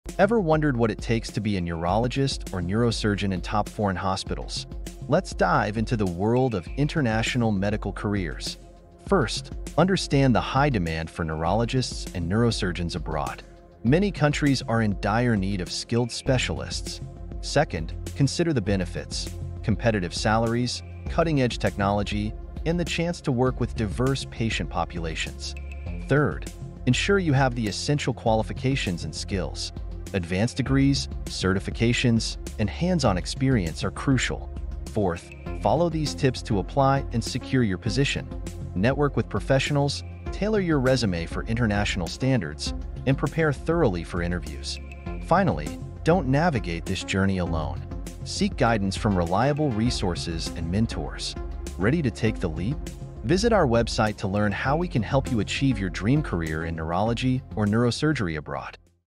Neurology and Neurosurgery Careers in Foreign Hospitals
Ever wondered what it takes to be a neurologist or neurosurgeon in top foreign hospitals? In this video, we explore the high demand for neurologists and neurosurgeons abroad, the benefits of pursuing a career in international medical fields, essential qualifications, application tips, and resources for guidance. Ready to take the leap?